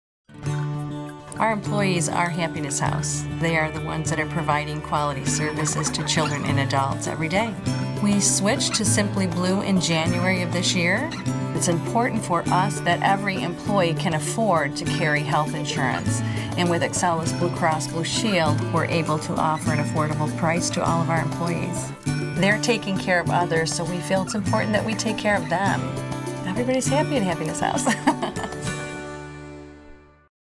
"Happiness House" :30 Excellus BlueCross BlueShield
Which health care plan do the people who specialize in caring for others choose for themselves? The one that offers the coverage they need at a price they can all afford. SimplyBlue from Excellus BlueCross BlueShield.

Director: Mike Baron
DP: James Gribbins
Production/Post; dPost, Buffalo